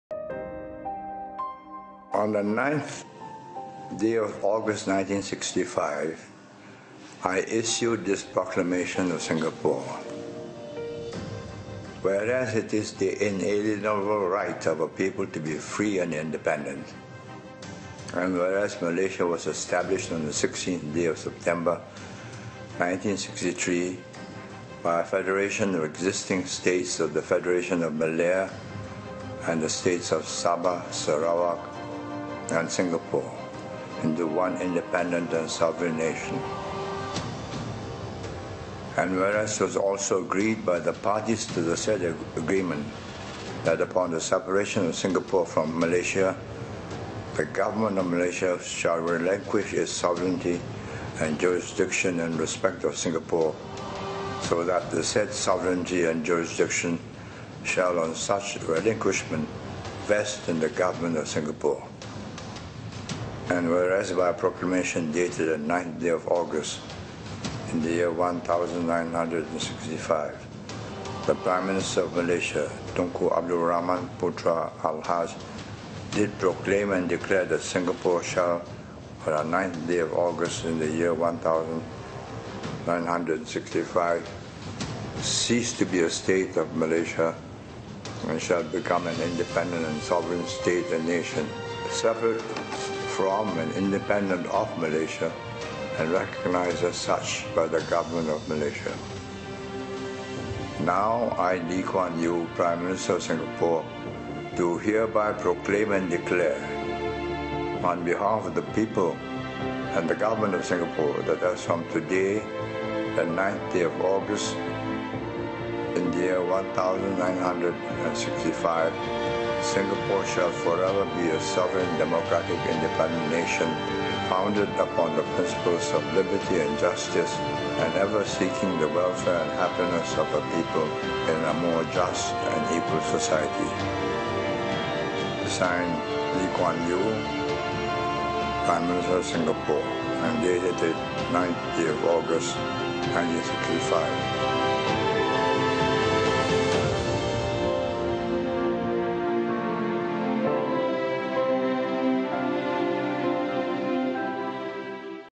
1965 李光耀發布新加坡獨立宣言
李光耀（1923年9月16日－2015年3月23日），客家人，祖籍廣東梅州。李光耀長期擔任新加坡共和國總理和人民行動黨秘書長，對新加坡歷史、經濟、政治均有影響，因此被稱為新加坡建國之父，也被美國《商業內幕》評為「20世紀最成功的獨裁者」第二名。